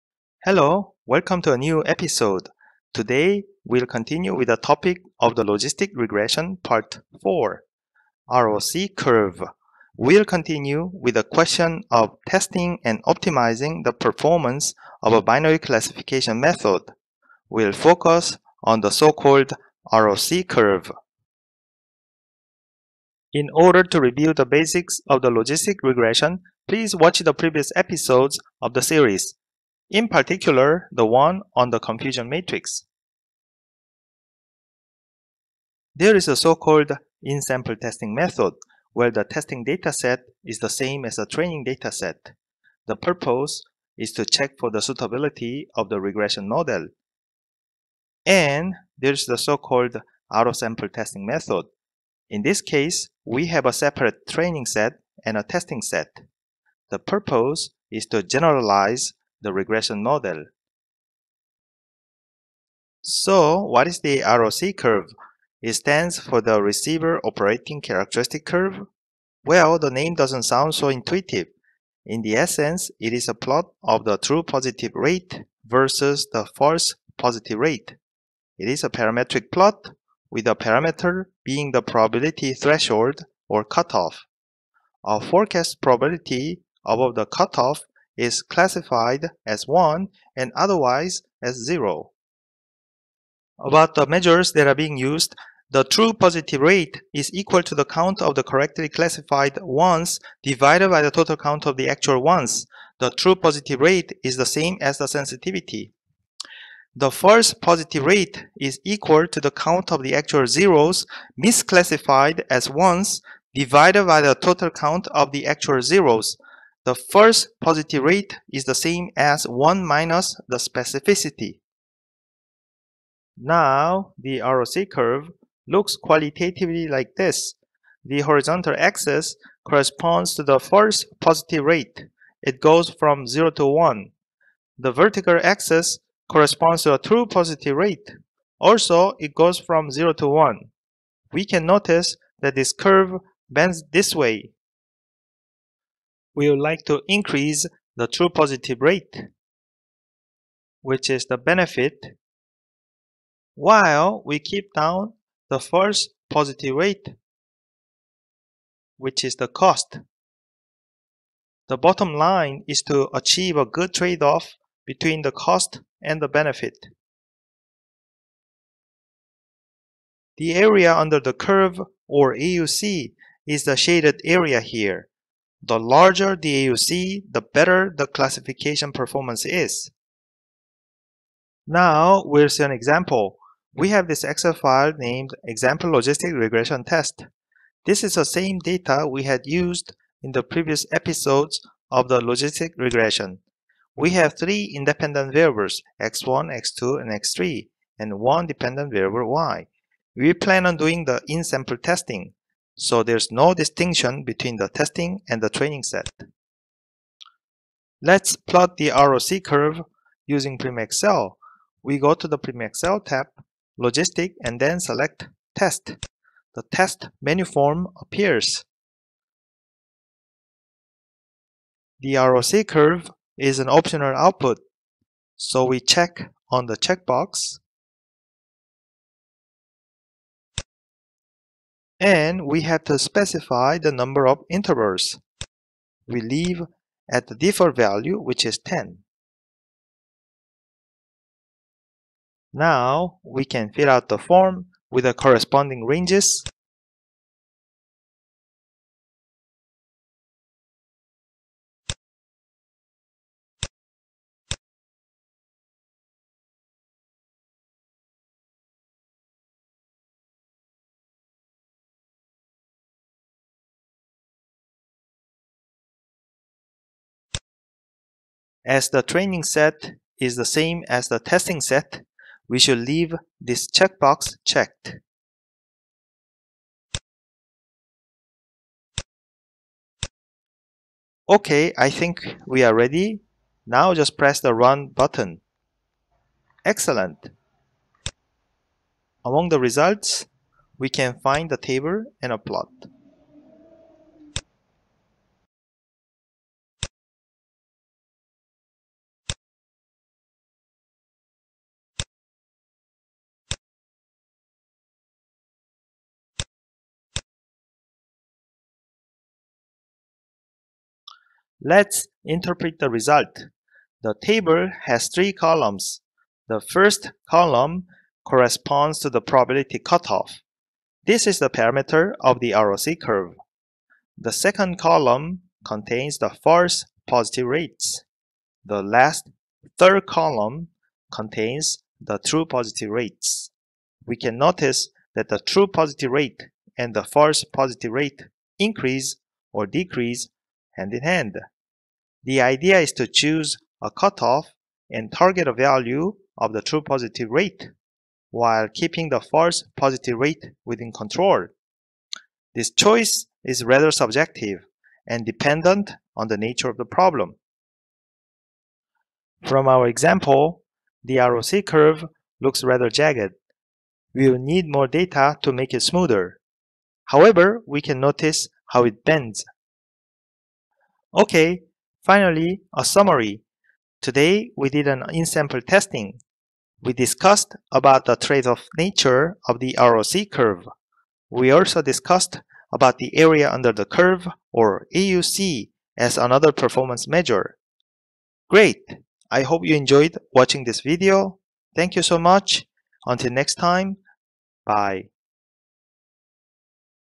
Learn Logistic Regression #4 : ROC Curve
In this episode, we show how to plot an ROC curve in Excel with the help of PrimaXL, an add-in software.